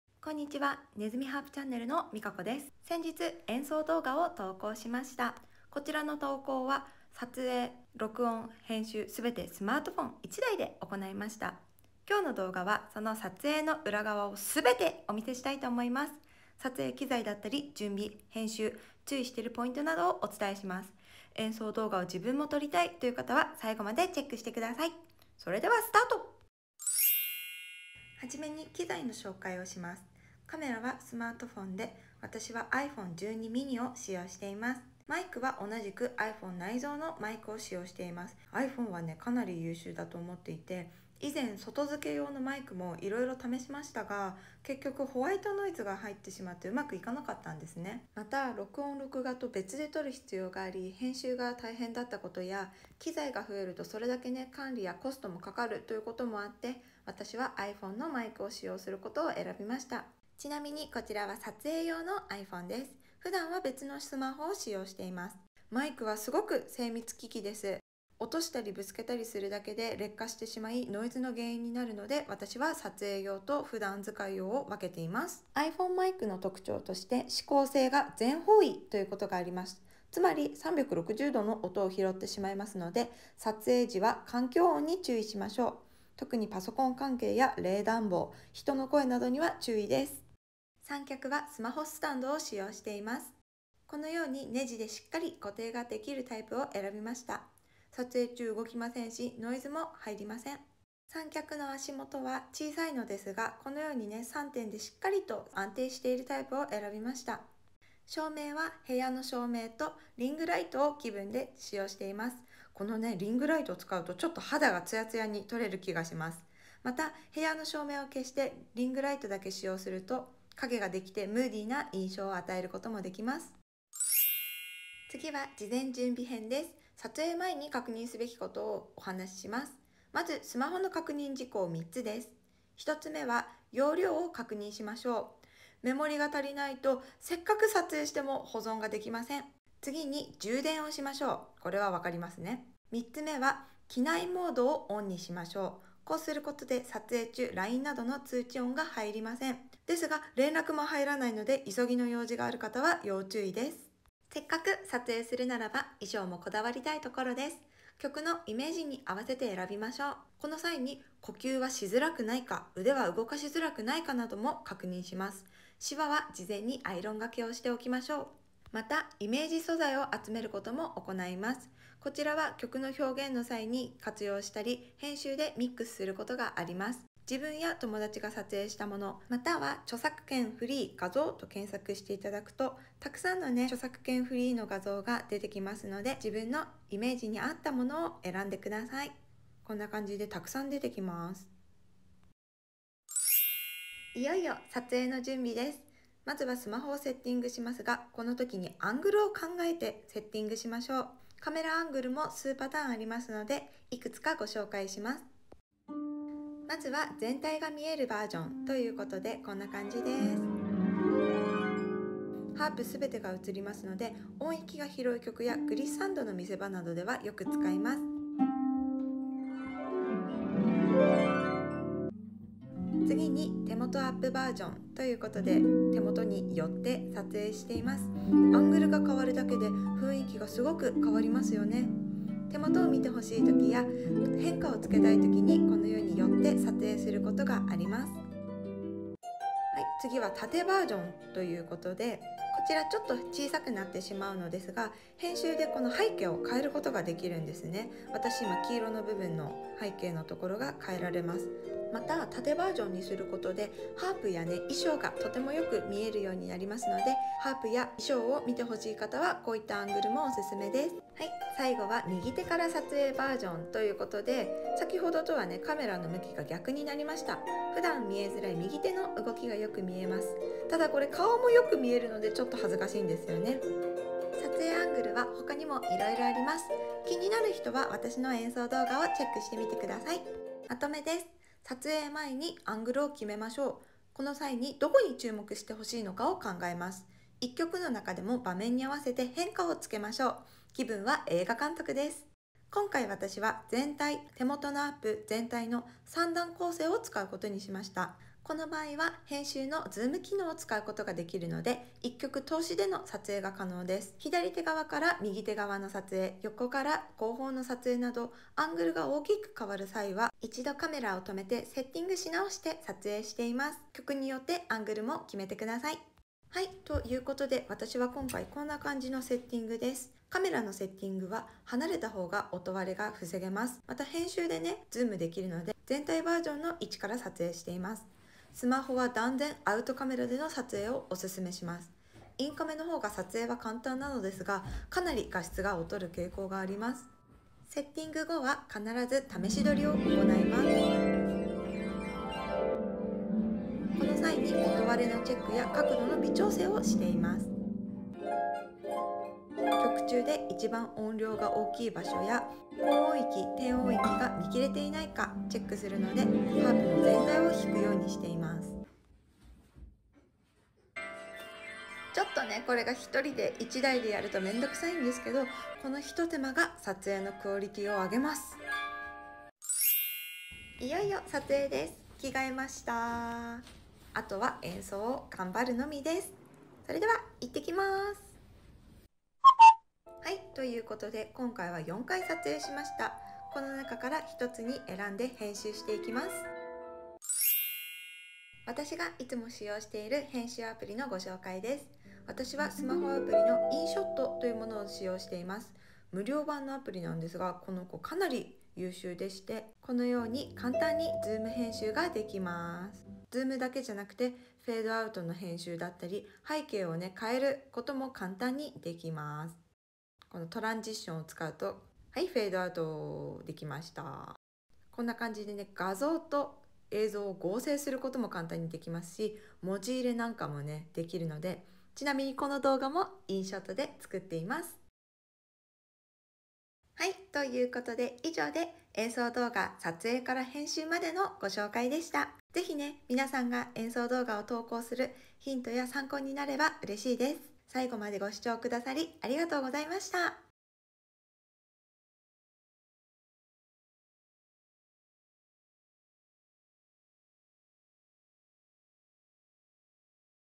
【解説】スマホ1台演奏動画の作り方/撮影から編集まで
ねずみハープチャンネル🐭のみかこです❣️

先日撮影した演奏動画の裏側を撮影しました😏✨
所々、私が工夫しているポイントを
惜しみなくご紹介しました💖

弾いてみた動画を撮りたい！編集したい！
と言う方の参考、ヒントになれば嬉しいです。

ミヤビメソードを習っています
師範免状獲得 第一号💮秋田支部

目次
00:00はじめに
00:27撮影機材
02:05事前準備
03:13撮影前の準備
03:18∟カメラアングルについて
05:18∟セッティング
06:10演奏&選定
06:27編集
07:07おわりに

練習風景や写真、練習してて思ったことなど書いてるよ(^^)